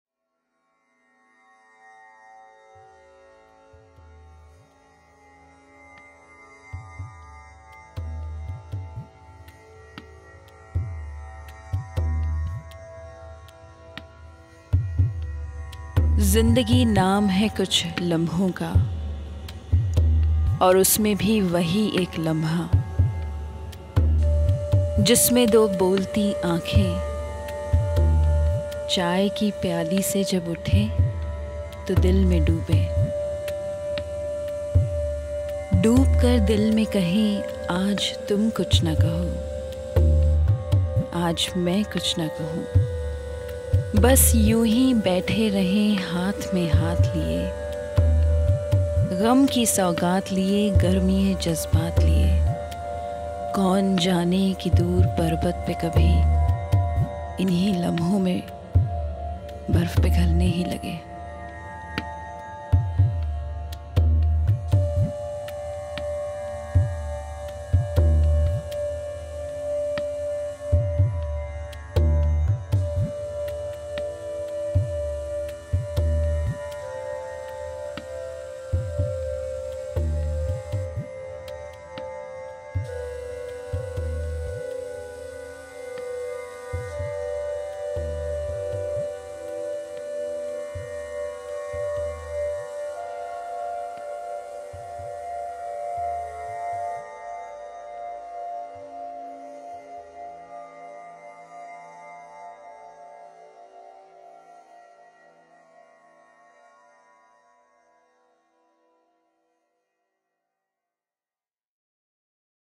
Sunset Poem
Sunset by Kafi Azmi

Messing making a bit of music in my little studio this morning that I use to record my meditations and just for fun I put this together featuring Abha reading Kafi Azmi's final poem to his wife written to her on the day of his passing, music is himself of course... and I hope you enjoy...

Love
John